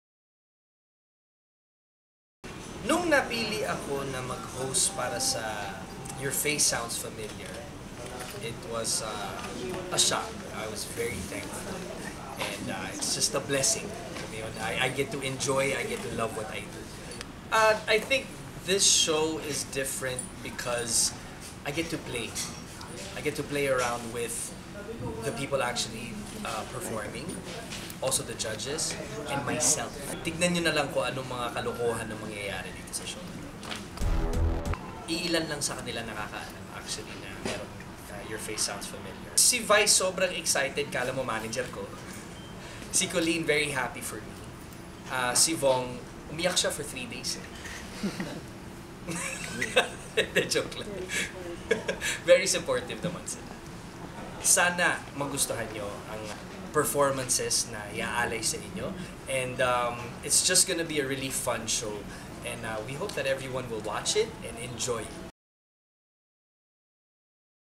Billy excited sa pagiging host ng Your Face Sounds Familiar
Ibinahagi ng International RNB star na si Billy Crawford ang kanyang pagka-excite sa hosting stint sa pinakabagong talent reality show ng ABS-CBN na "Your  Face Sounds Familiar"